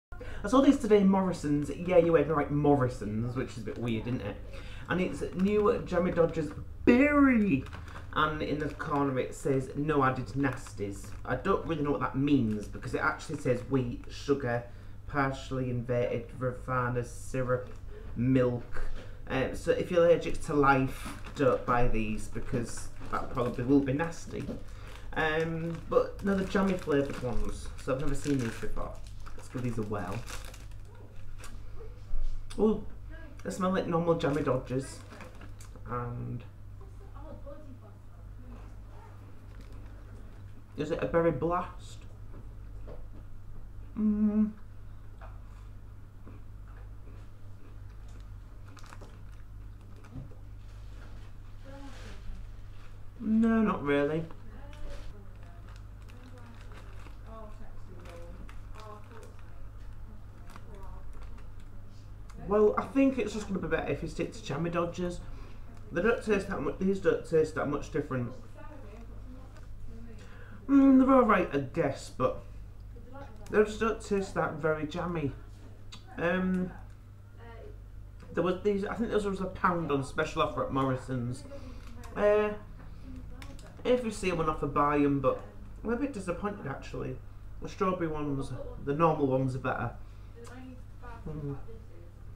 Jammie Dodgers Berry Blast FIRST REVIEW
A new version on Jammie Dodgers was in Morrisons so I thought I would review them.